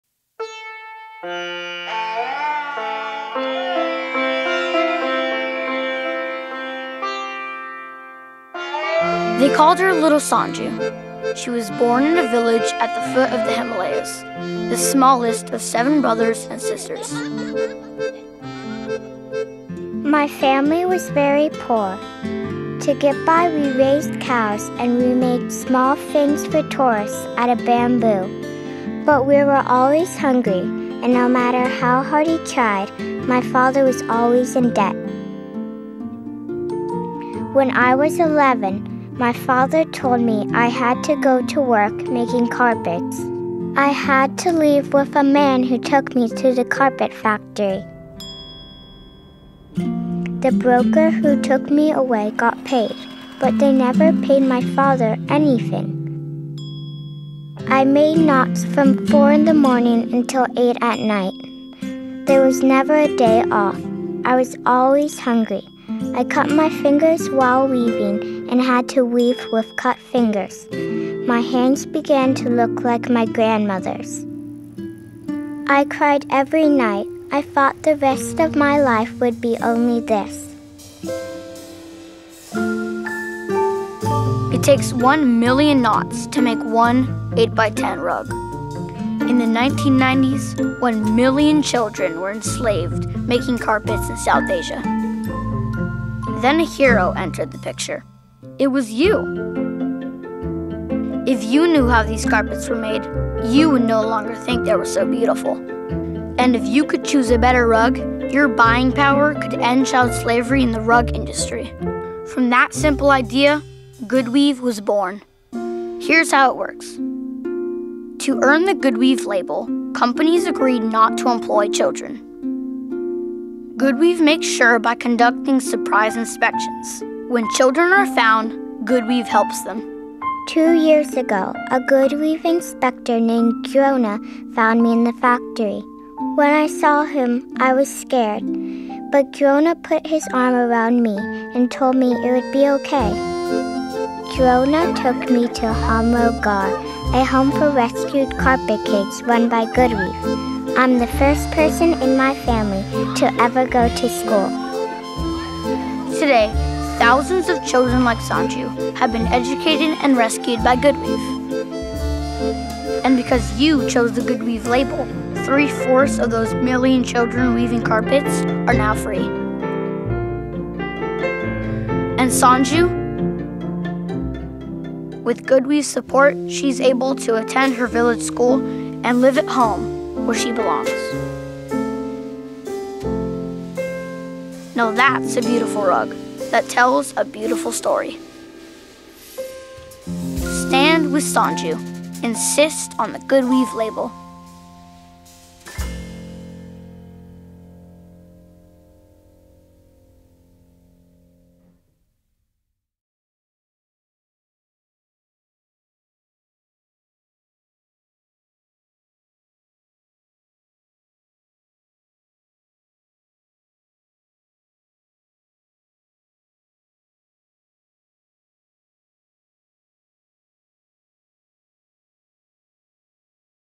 Stand with Sanju
GoodWeave.org/Sanju

Child servitude is a crime committed against 168 million children worldwide. Stand With Sanju demonstrates what an important role consumers play in making a difference. By using a rigorous and internationally recognized certification program, GoodWeave certifies carpet supply chains as being free of child labor. Additionally, they provide support for children like Sanju who have been rescued.

With your help, we can end child labor by 2020. Be a part of the movement. Take action now.